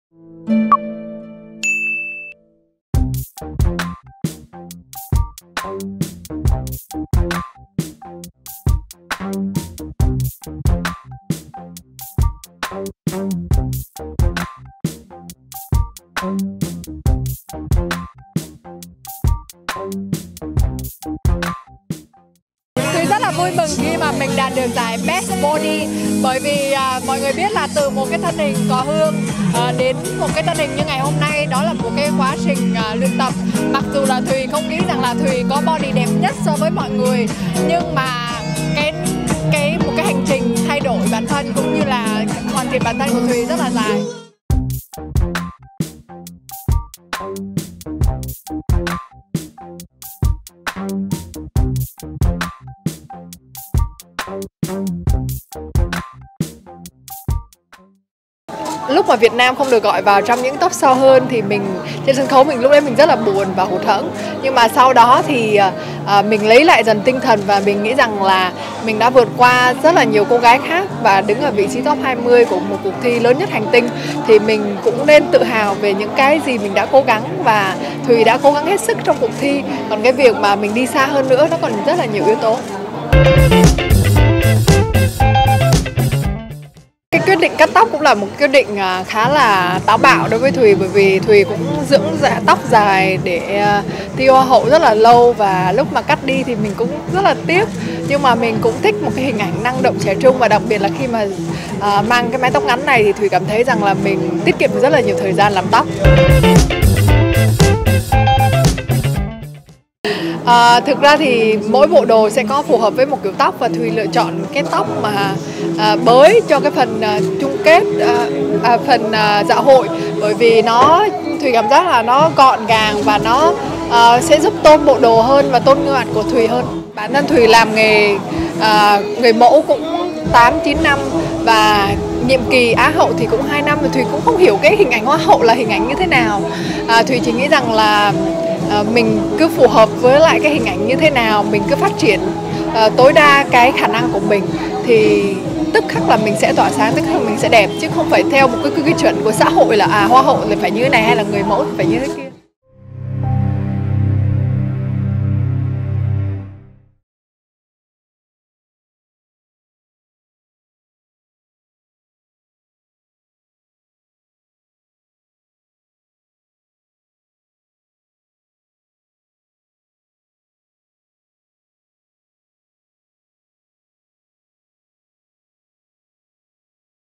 Hoàng Thùy diện váy cúp ngực nhận giải 'Best body of the year'
Bên cạnh Hoàng Thùy, Tiểu Vy đã nhận được Best face of the year, Song Luân đã nhận được giải Best looking gentleman và Salim đã nhận được giải Best hair tại giải thưởng Elle beauty adwards 2020. Ngoài ra còn có hàng loạt các hạng mục trao giải cho các mỹ phẩm làm đẹp.
Theo dõi video để lắng nghe thêm những chia sẻ từ Á hậu Hoàng Thùy.